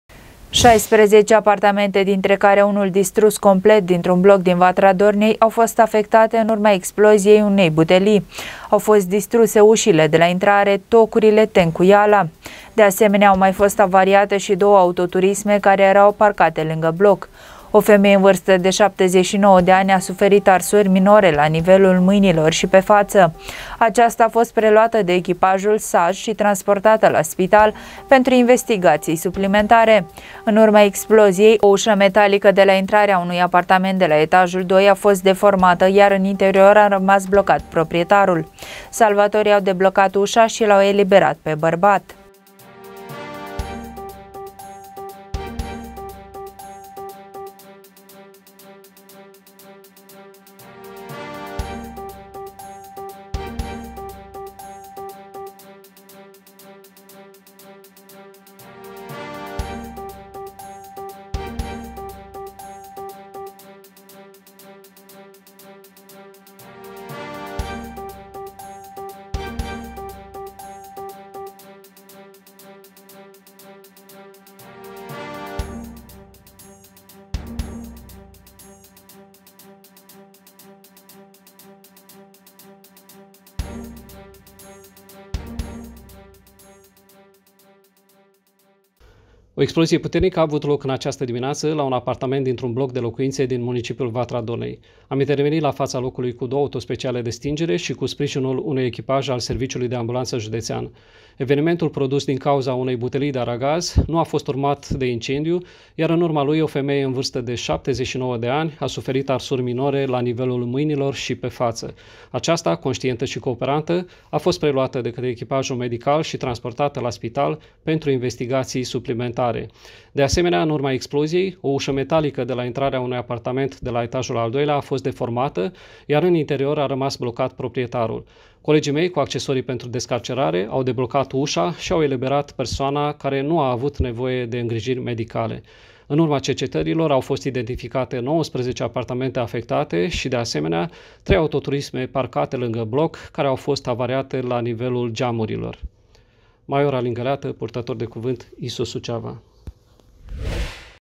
16 apartamente afectate în urma exploziei unei butelii
16 apartamente, dintre care unul distrus complet, dintr-un bloc din Vatra Dornei au fost afectate în urma exploziei unei butelii. Au fost distruse ușile de la intrare, tocurile, tencuiala. De asemenea, au mai fost avariate și două autoturisme care erau parcate lângă bloc. O femeie în vârstă de 79 de ani, a suferit arsuri minore la nivelul mâinilor și pe față. Aceasta a fost preluată de echipajul SAJ și transportată la spital pentru investigații suplimentare. În urma exploziei, o ușă metalică de la intrarea unui apartament de la etajul II a fost deformată, iar in interior a rămas blocat proprietarul. Salvatorii au deblocat ușa și l-au eliberat pe bărbat.